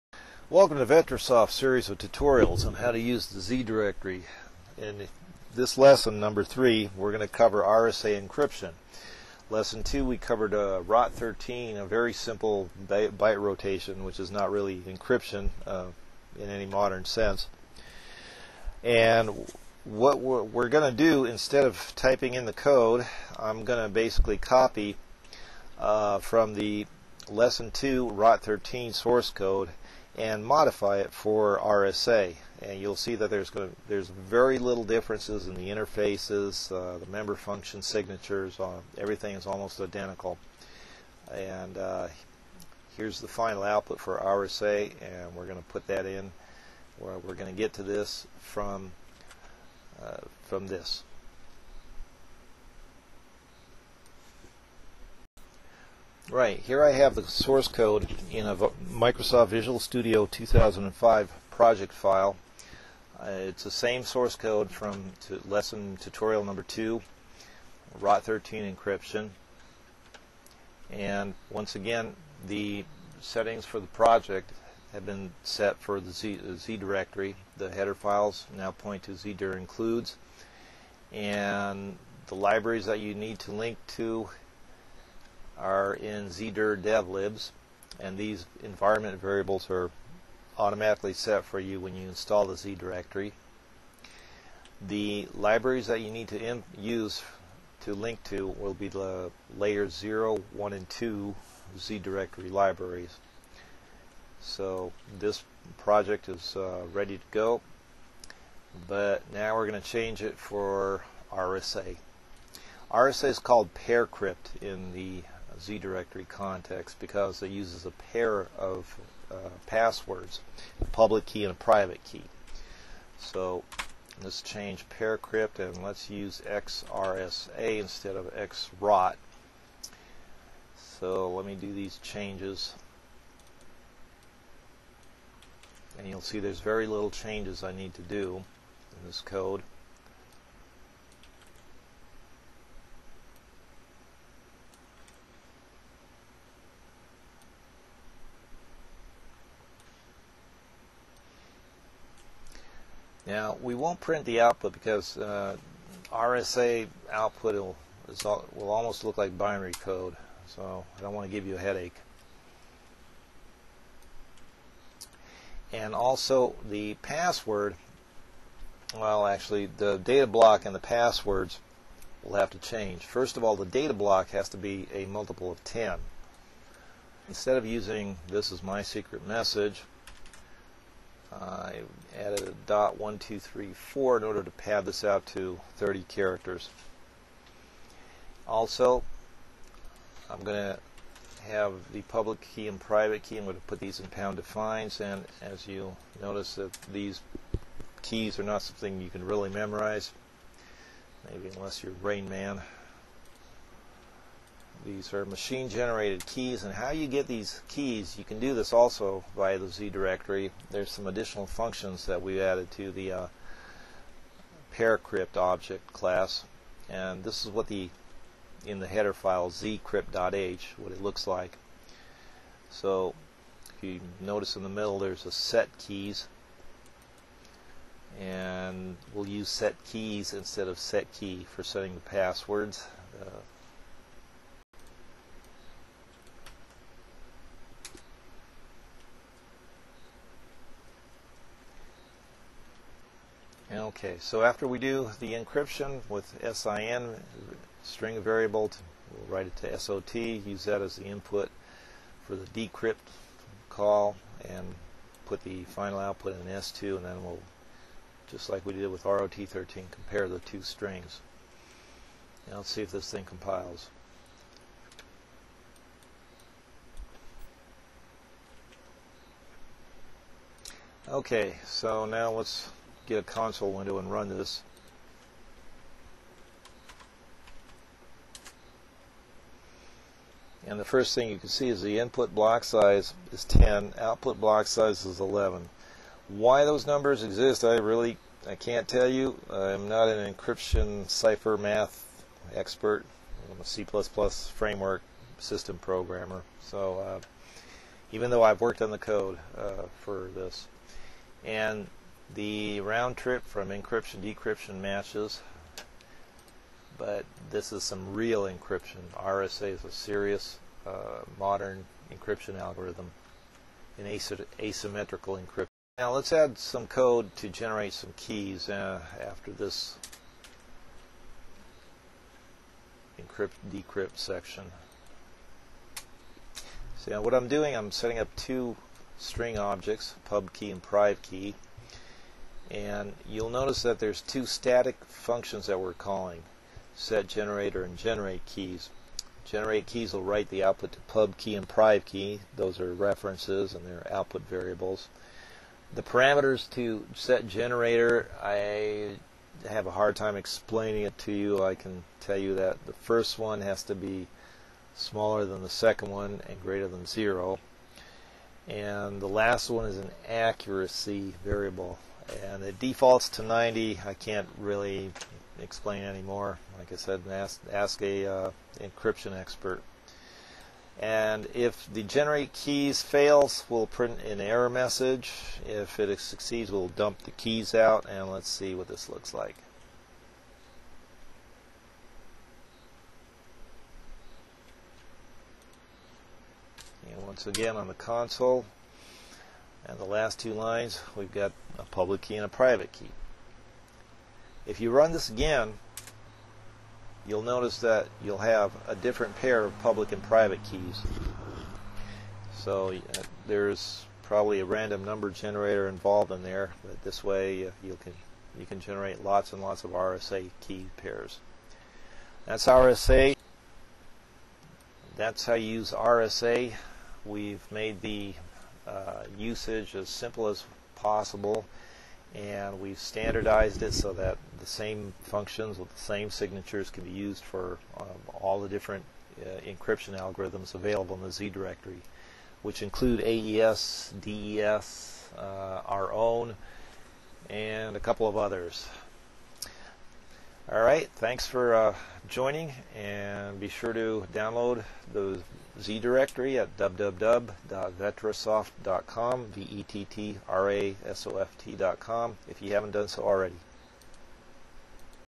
[ZDir-C++ #3]: advanced RSA encryption, using C++ with the Z DIrectory (tutorial 3)
Here we see that although the underlying cipher algorithm is very different from that in video #2 (ROT-13), the source code is almost identical. Z Directory interfaces have been made as simple and standardized as possible. RSA is a popular, modern, heavy-duty encryption algorithm.